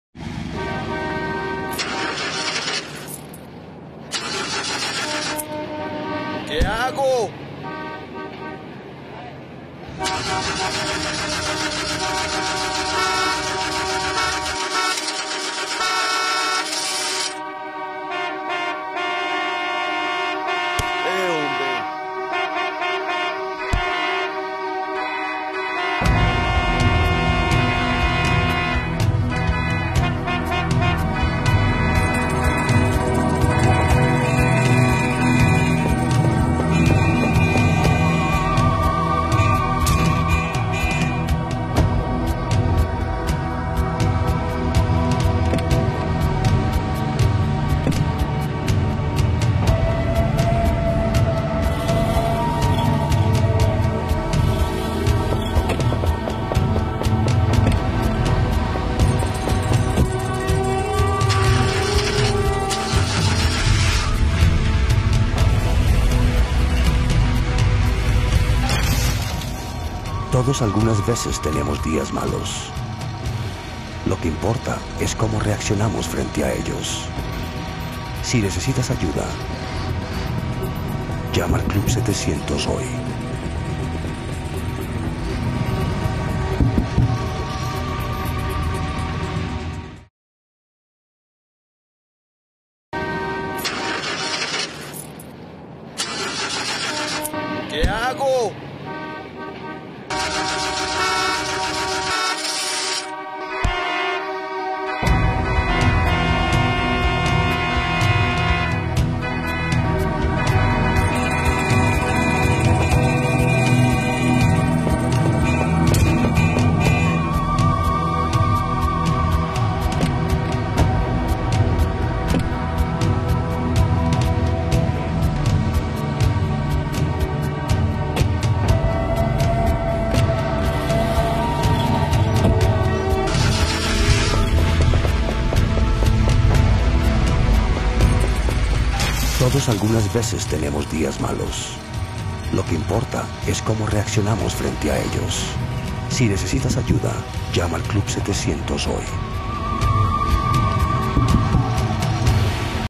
Traffic Jam. When everything seems going wrong.
A spot to realize that all the times the troubles seems bigger than us. But what do we can do to face them!
Directed and Photographed by Juan Cajiao as Associated Producer for CBN